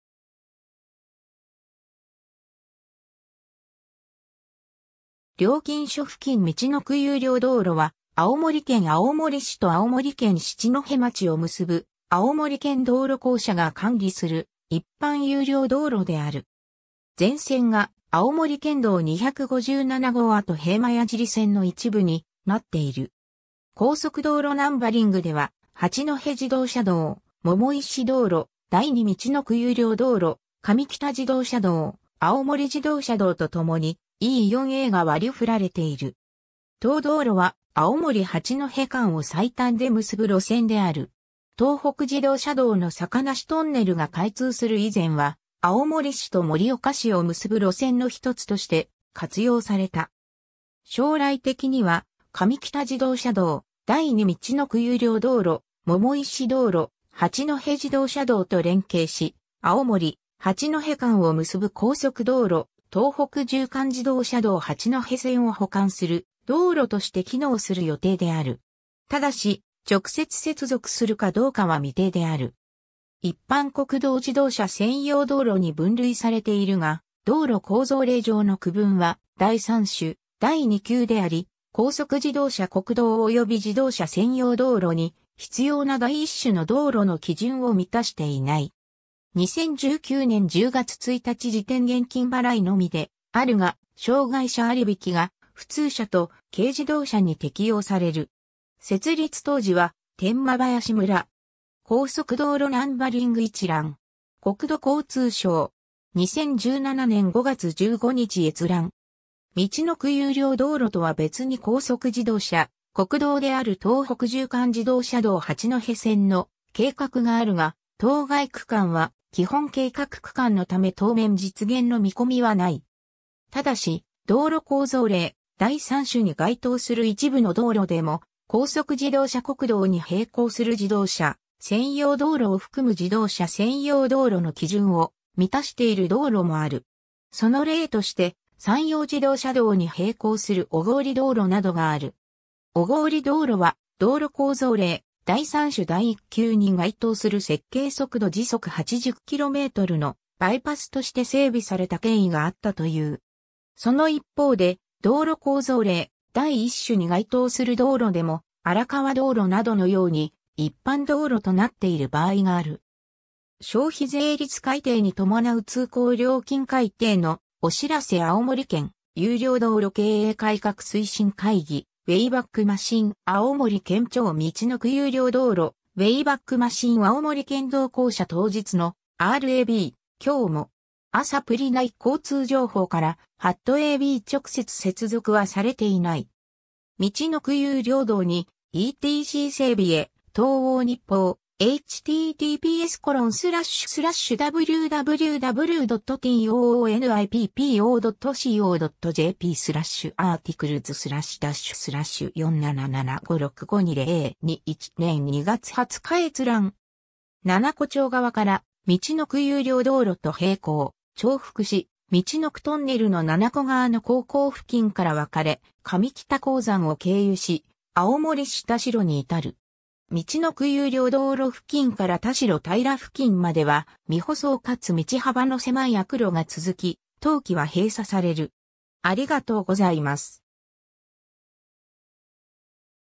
みちのく有料道路
料金所付近 みちのく有料道路（みちのくゆうりょうどうろ）は、青森県青森市と青森県七戸町を結ぶ青森県道路公社が管理する一般有料道路である。
全線が青森県道257号後平馬屋尻線の一部になっている。
 高速道路ナンバリングでは、八戸自動車道、百石道路、第二みちのく有料道路、上北自動車道、青森自動車道とともに「E4A」が割り振られている。
 当道路は青森 - 八戸間を最短で結ぶ路線である｡ 東北自動車道の坂梨トンネル（小坂IC - 碇ヶ関IC間）が開通する以前は、青森市と盛岡市を結ぶ路線の一つとして活用された。
 将来的には、上北自動車道（天間林道路、上北天間林道路、上北道路）、第二みちのく有料道路、百石道路、八戸自動車道と連携し、青森・八戸間を結ぶ高速道路東北縦貫自動車道八戸線を補完する道路として機能する予定である。
ただし、直接接続するかどうかは未定である。
 一般国道自動車専用道路(A')に分類されているが、道路構造令上の区分は第3種第2級であり、高速自動車国道及び自動車専用道路に必要な第1種の道路の基準を満たしていない。
 2019年10月1日時点 現金払いのみであるが、障害者割引が普通車と軽自動車に適用される。
 ^ 設立当時は天間林村。
 ^ “高速道路ナンバリング 一覧”. 国土交通省. 2017年5月15日閲覧。
 ^ みちのく有料道路とは別に高速自動車国道である東北縦貫自動車道八戸線の計画があるが、当該区間は基本計画区間のため当面実現の見込みはない。
 ^ ただし、道路構造令第3種に該当する一部の道路でも、高速自動車国道に並行する自動車専用道路を含む自動車専用道路の基準を満たしている道路もある。
その例として、山陽自動車道に並行する小郡道路（国道2号バイパス）などがある。
小郡道路は道路構造令第3種第1級に該当する設計速度時速80キロメートルのバイパスとして整備された経緯があったという。
その一方で、道路構造令第1種に該当する道路でも、荒川道路（新潟山形南部連絡道路の一部を構成する）などのように一般道路となっている場合がある。
 ^ 消費税率改定に伴う通行料金改定のお知らせ ^ 青森県有料道路経営改革推進会議 - ウェイバックマシン（2011年3月23日アーカイブ分） - 青森県庁 ^ みちのく有料道路 - ウェイバックマシン（2011年8月21日アーカイブ分） - 青森県道公社 ^ 当日のRAB「今日も!あさぷり」内交通情報から ^ a b 直接接続はされていない。
^ みちのく有料道にETC整備へ, 東奥日報, (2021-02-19), 2021年2月20日閲覧。
 ^ 七戸町（旧天間林村）側からみちのく有料道路と並行・重複し、みちのくトンネルの七戸側の坑口付近から分かれ、上北鉱山を経由し青森市田代に至る。
みちのく有料道路付近から田代平付近までは未舗装かつ道幅の狭い悪路が続き、冬期は閉鎖される。